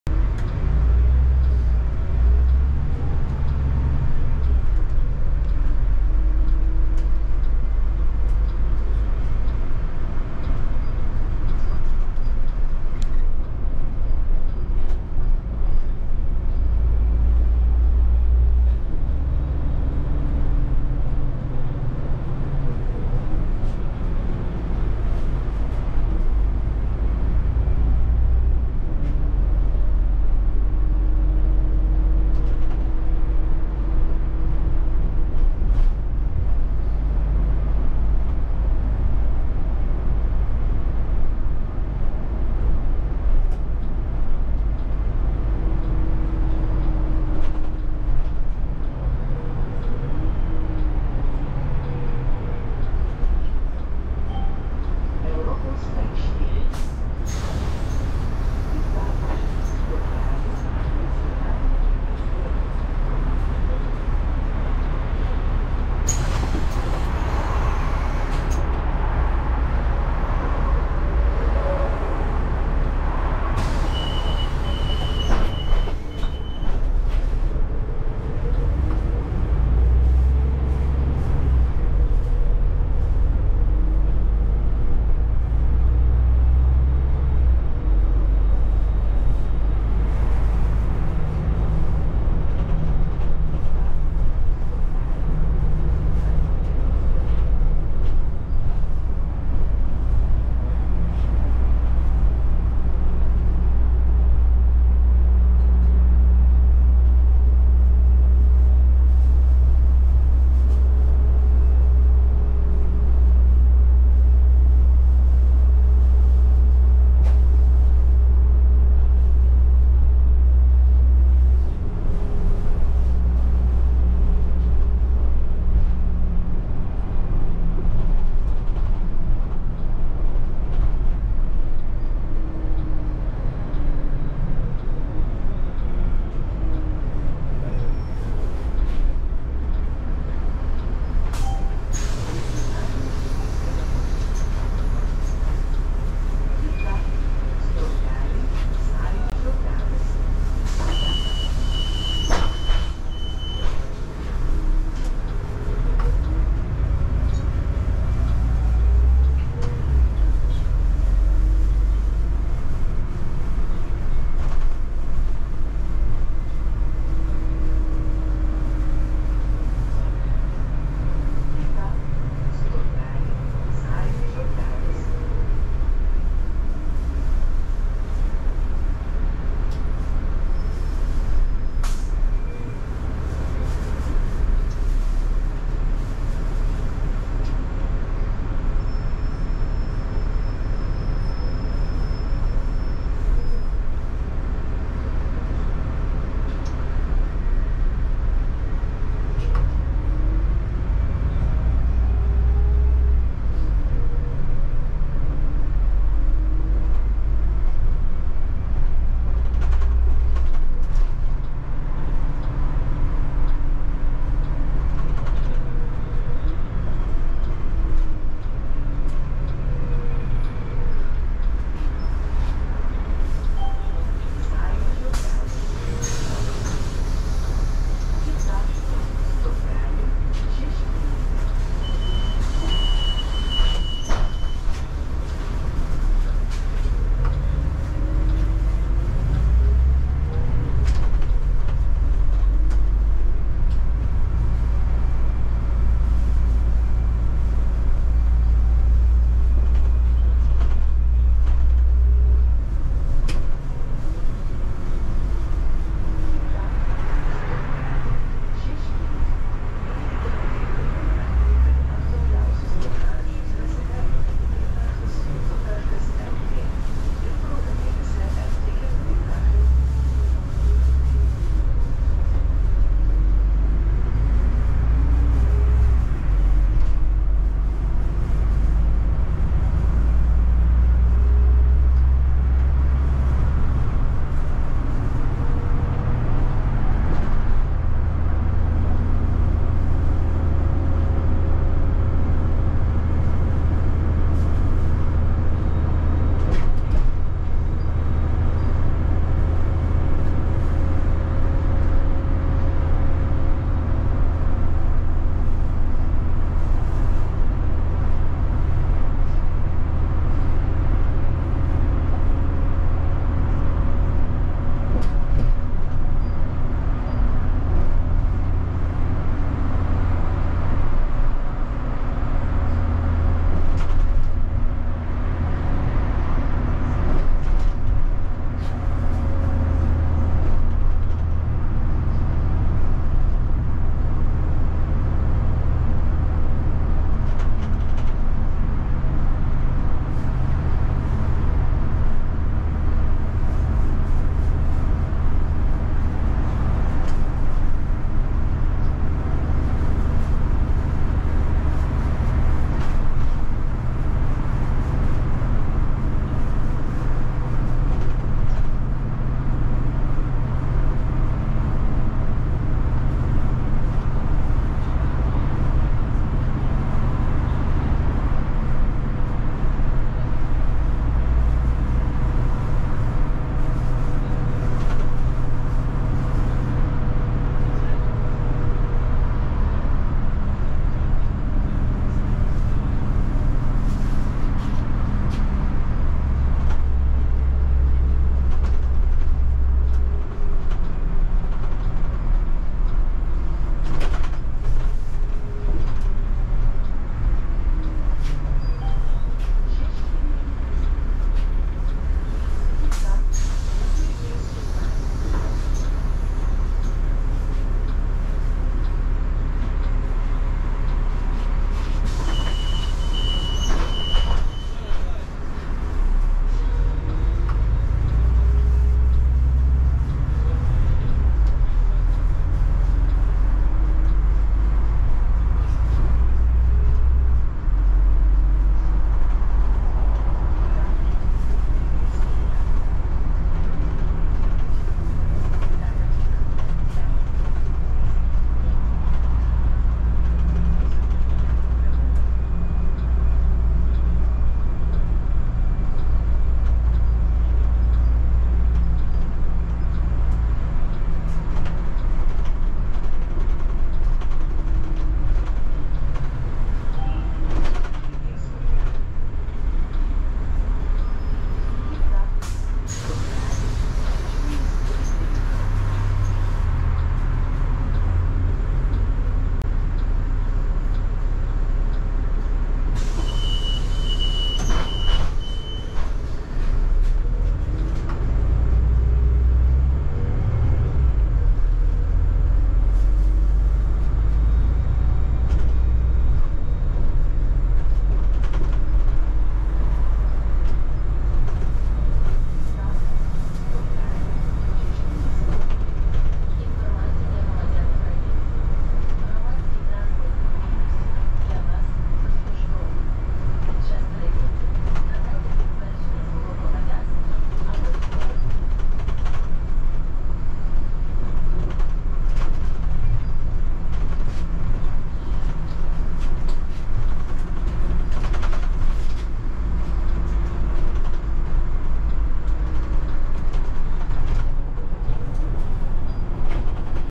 Riding bus in Vilnius in sunny autumn morning!
Let's take a bus and have a ride in Vilnius through Europos street uphill to šeškinė. And enjoy some views and traffic through the city.